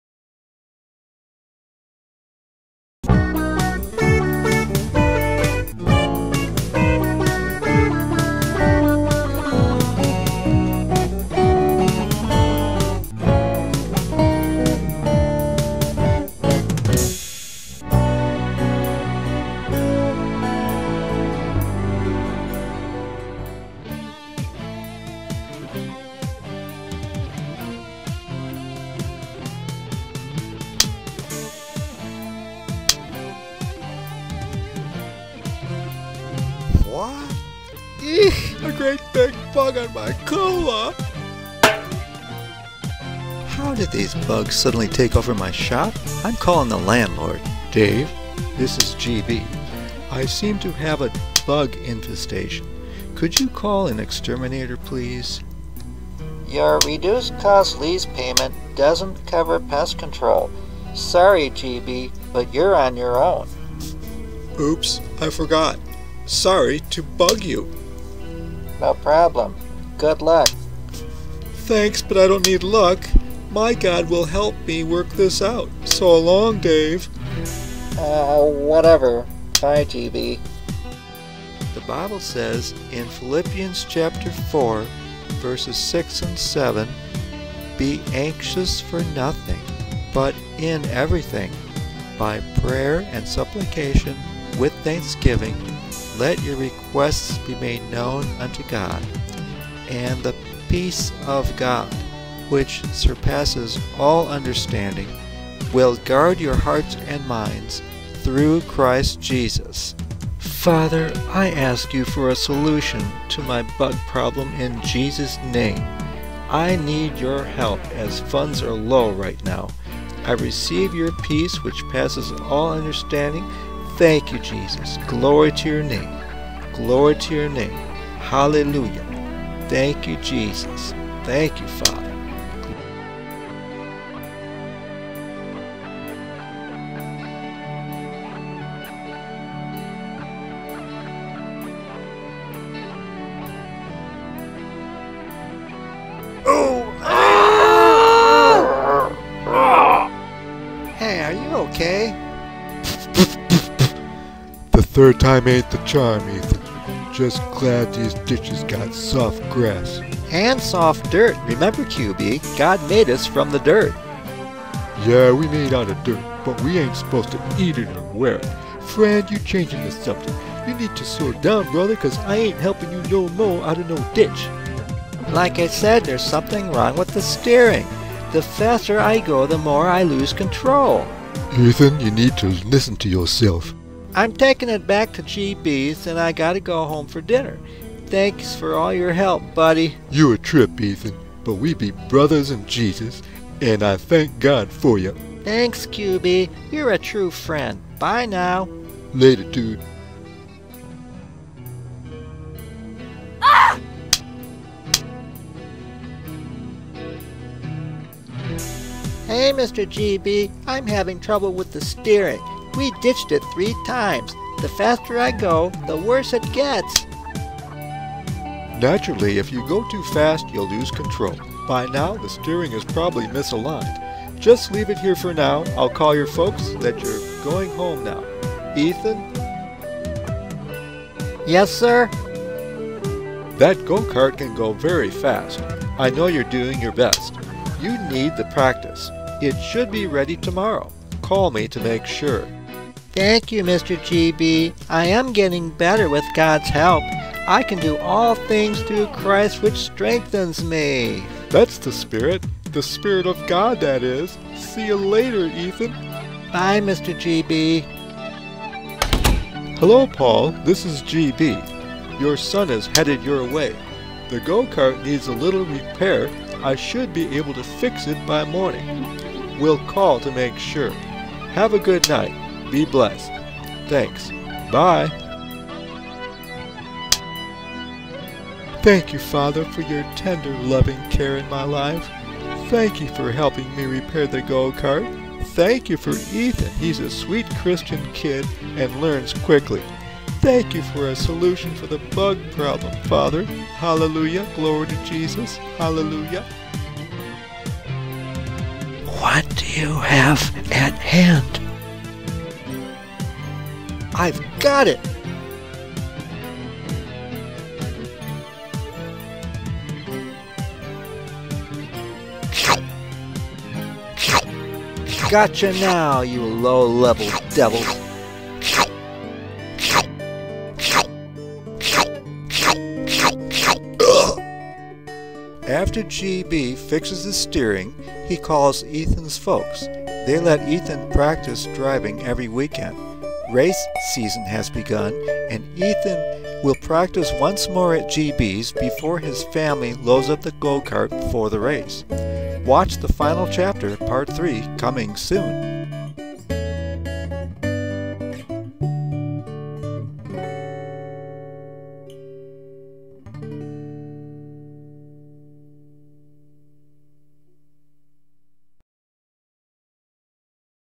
Ethan's Go-Cart Part 2
Obviously this is a continuation of Ethan's Go-Cart Part 1. Ethan and GB both have problems, but through God's help they learn or are learning to overcome them in Jesus' Name.